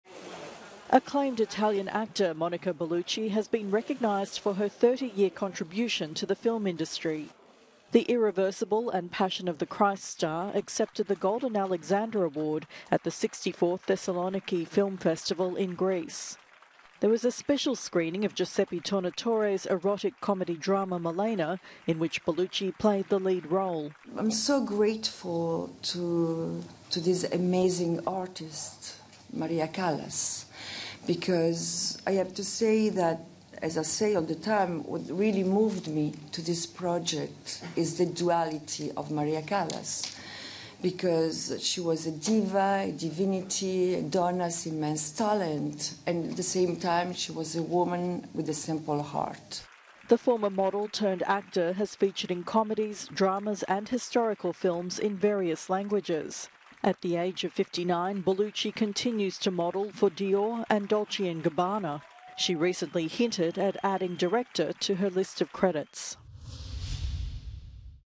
Monica Bellucci honoured for 30 years in film at Thessaloniki Film Festival
In a celebration of her three-decade-long career in the film industry, renowned Italian actress Monica Bellucci has been given the Golden Alexander Award at the 64th Thessaloniki Film Festival in Greece.